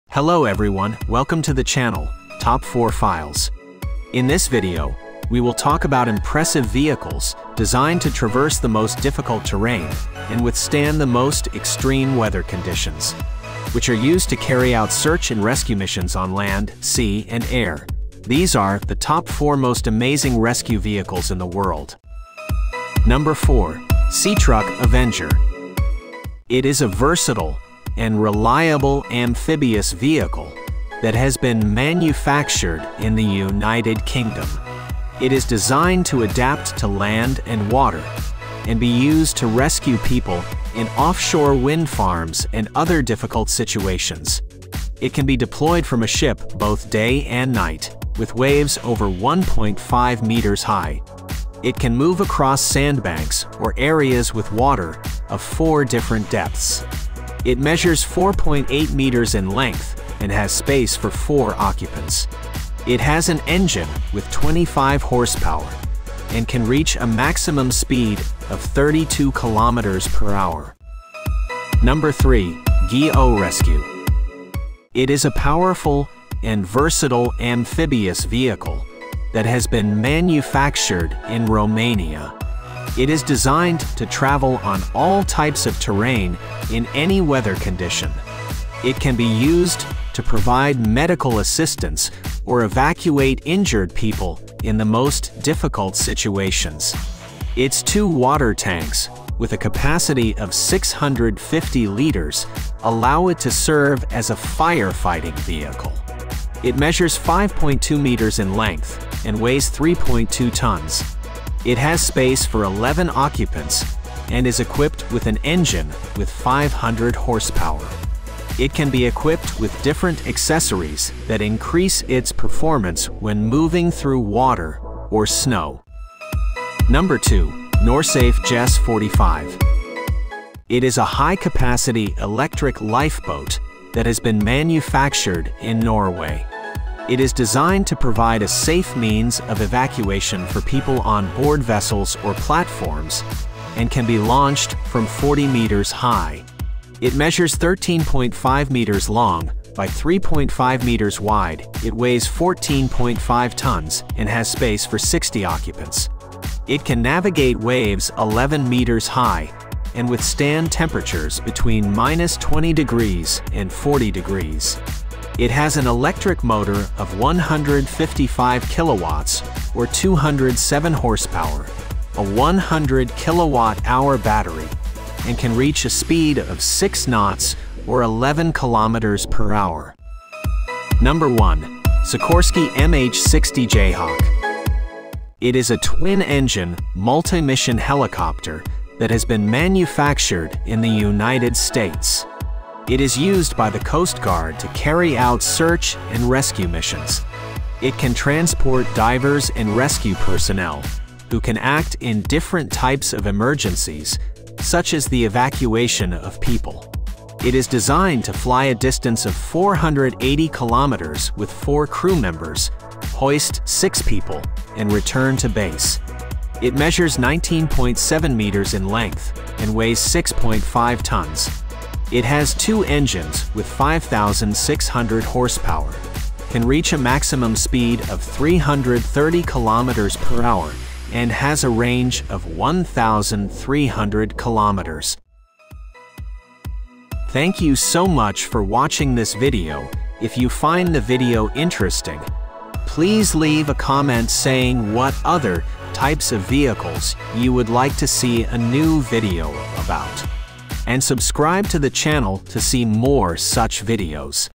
Top 4 Most Amazing Rescue Vehicles in the World | Amazing Technology | Top 4 Files
Rescue vehicles such as boats, off-road cars, helicopters or planes

4x4 vehicles, amphibious vehicles and others emergency vehicles used for emergency evacuation or search and rescue at sea or in the mountains

emergency response vehicles designed to traverse difficult terrain with extreme weather conditions and rescue people in need of help

4. CTruk Avenger

3. Ghe-O Rescue

2. Norsafe GES 45

1. Sikorsky MH-60 Jayhawk